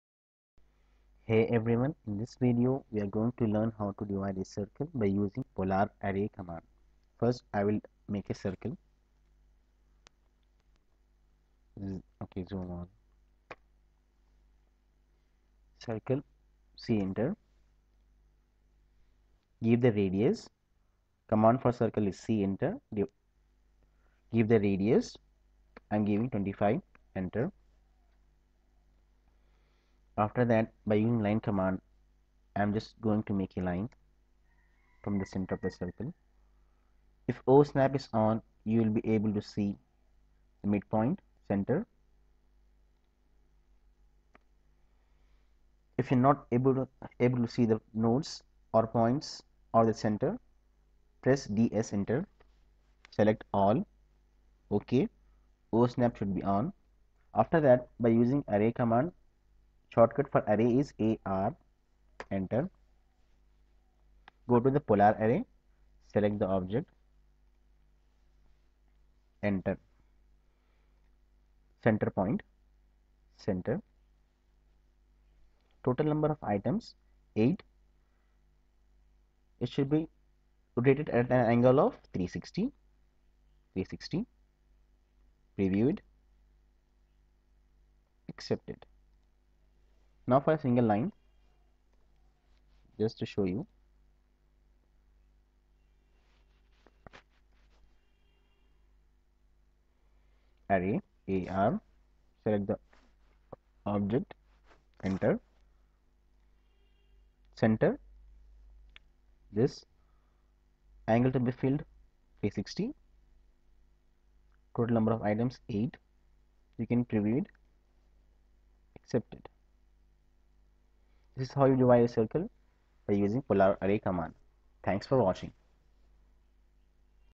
polar array how to divide a circle in Auto CAD
A tutorial on how to divide a circle by using array command in Auto CAD